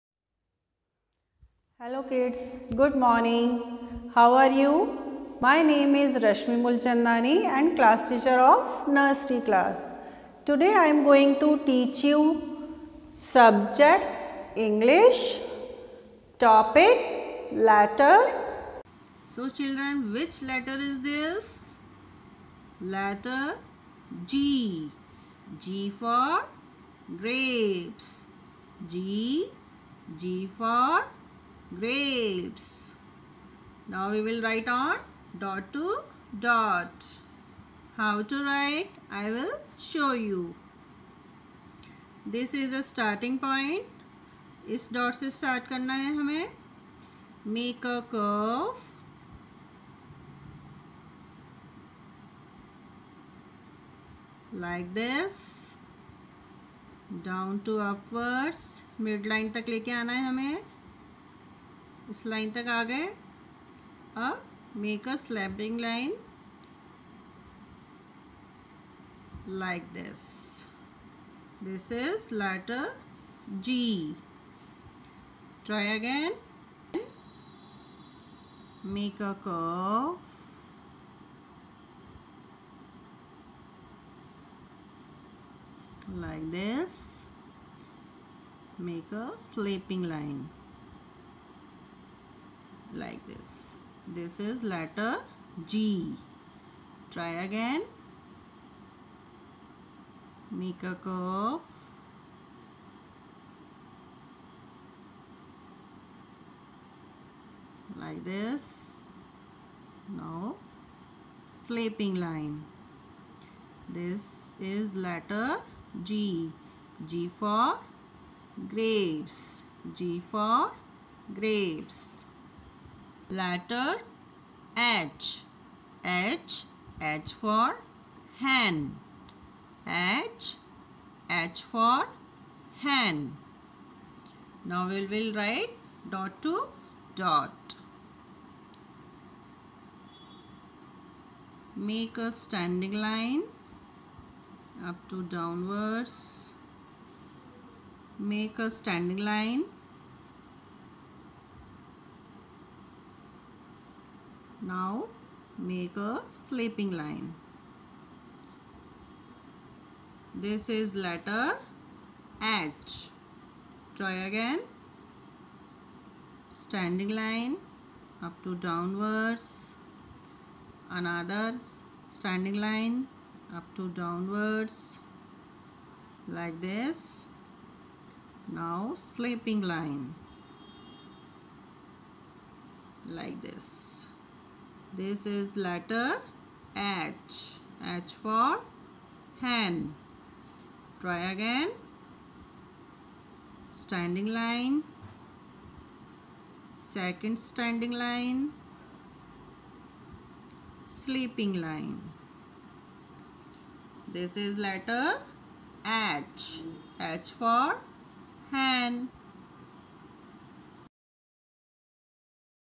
CLASS-NURSERY_SUBJECT-ENGLISH_TOPIC-LETTERS-G and H
In this video you will learn about the formations of the letters.